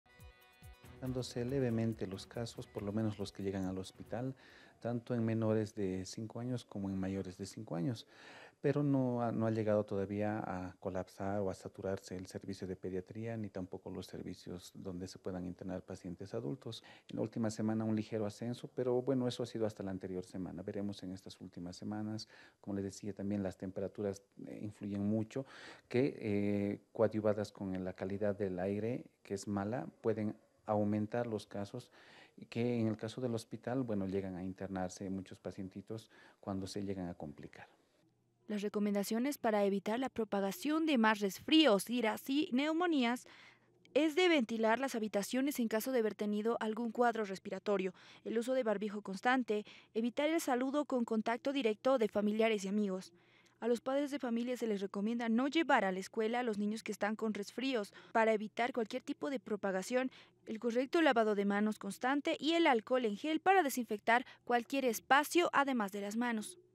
Piden uso de barbijos, desinfectantes, ventilación de habitaciones.
Piden uso de barbijos, desinfectantes, ventilación de habitaciones y evitar saludo con contacto directo.